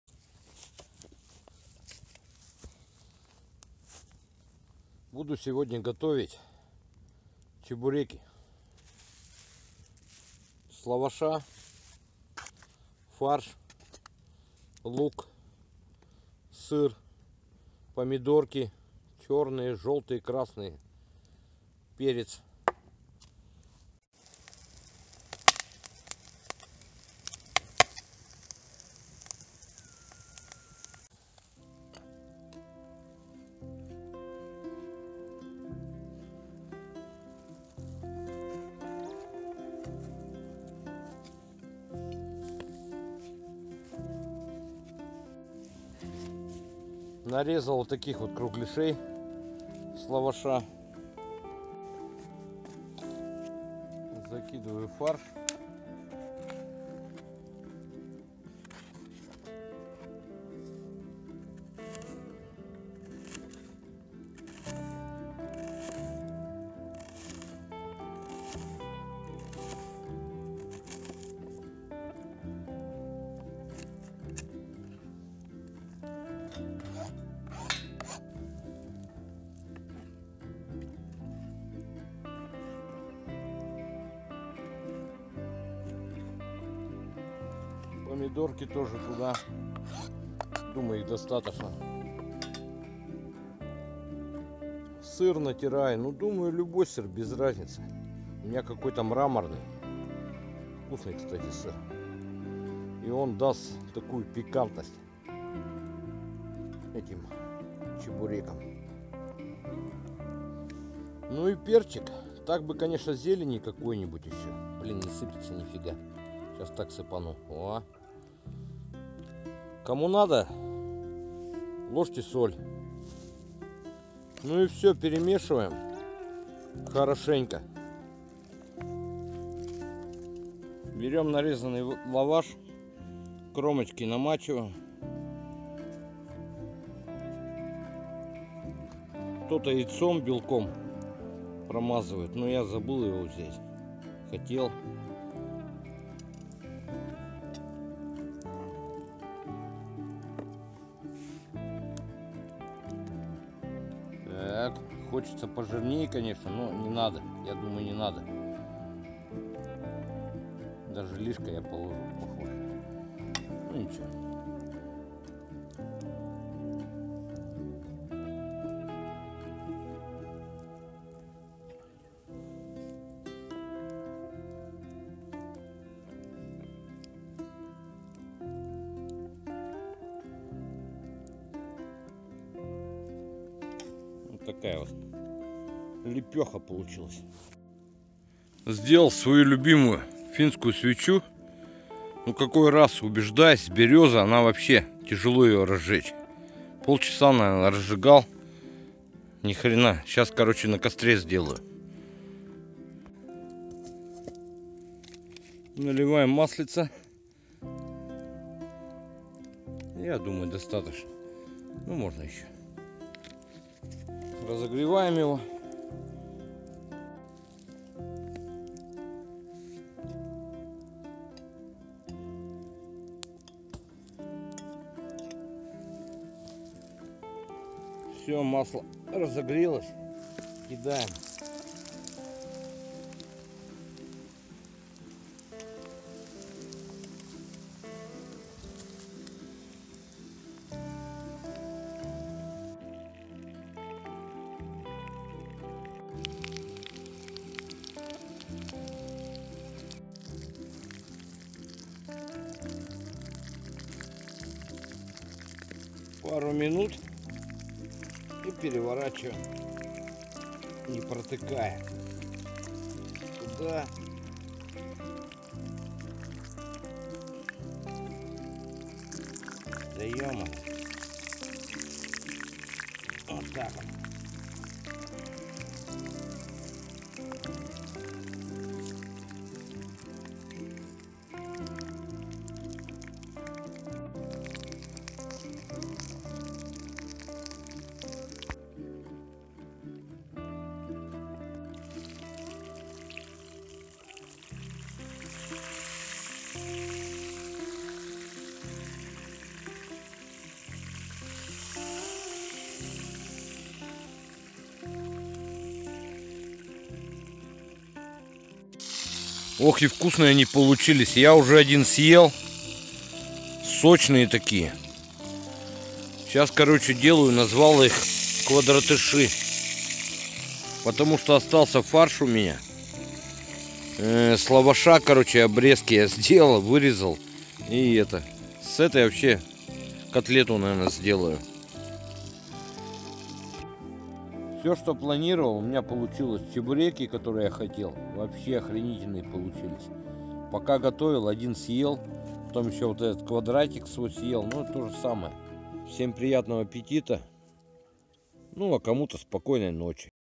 Чебуреки из лаваша на костре!!!
Очень вкусный рецепт,я сам был неожиданно удивлен.Всем приятного аппетита!Поддержите подпиской,будет ещё много интересного!